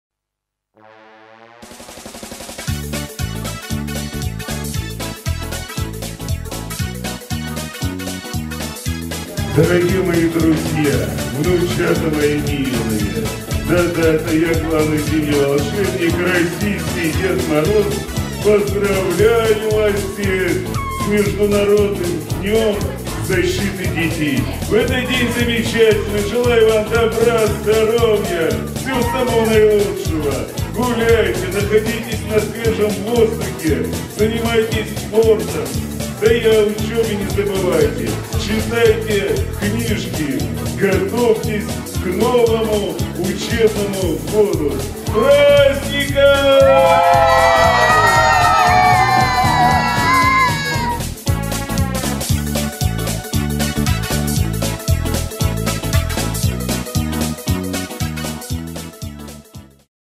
1 июня Международный День защиты детей 2017
Милые мои внучата!
Поздравляю вас с Праздником Детства - Международным Днем защиты детей!
Пусть лето принесет вам радость, веселье, встречу с новыми друзьями, новые открытия, путешествия и приключения! Отдохните как следует, наберитесь сил. Пусть всегда вам сияет солнышко и окружает тепло взрослых, которое подарит счастливое детство.
Ну, и меня не забывайте, все лето жду вас в гости на самые необыкновенные чудесные сказки.
С праздником, родные мои!